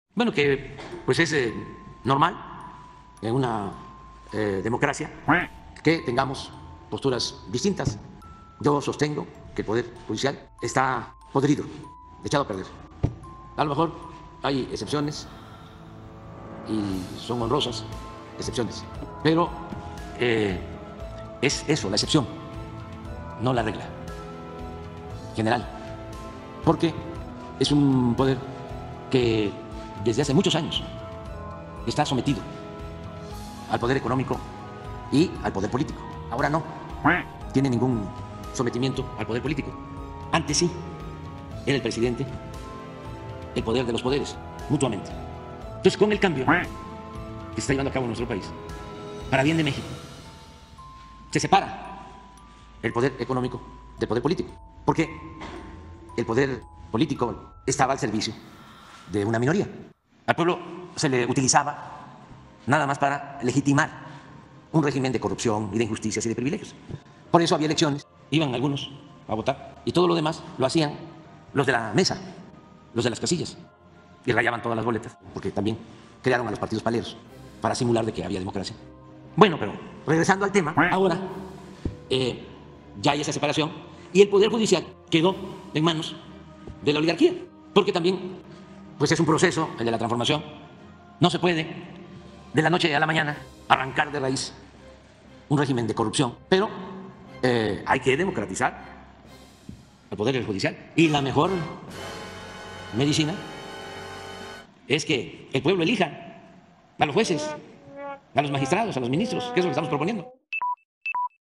El Poder Judicial está podrido 🦆, la mejor medicina es que el pueblo elija a los Ministros 🦆
Es normal en una democracia que tengamos posturas distintas, sostengo que el Poder Judicial está podrido 🦆, antes “no había democracia, era una oligarquía, ahora es distinto”, “la mejor medicina es que el pueblo elija a los Jueces, Ministros y Magistrados”